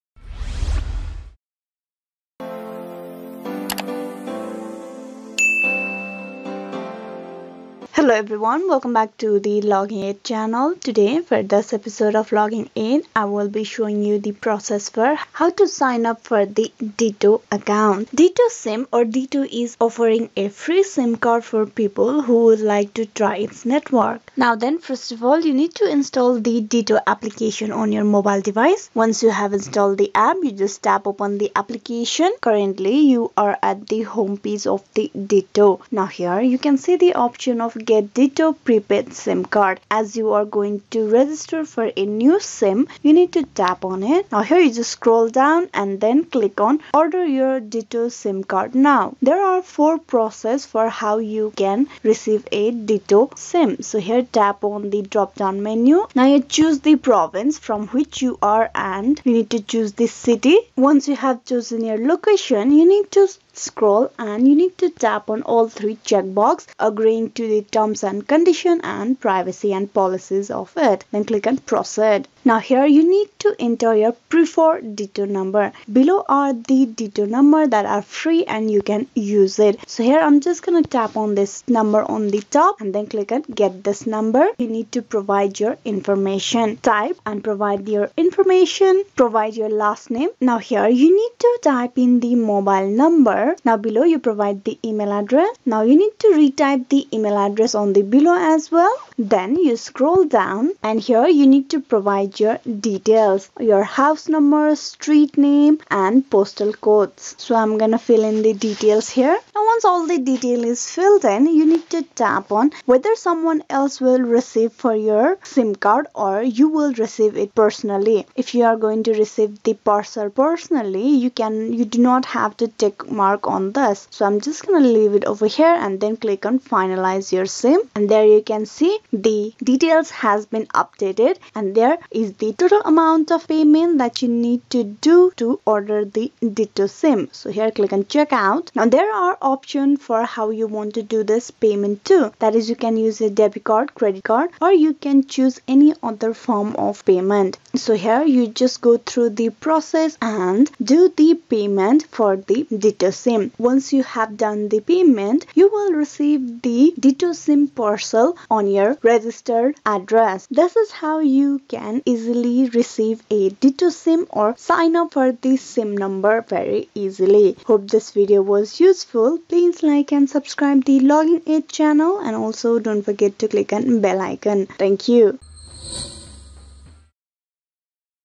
How To Order DITO Sim Card 2022 | DITO Sign Up Help | Get DITO Sim Card
DITO is offering free SIM card to the people who would like to try its network.
Get enrolled in DITO account in a easy steps. Watch this video till the end to let know on how you can register to DITO Account.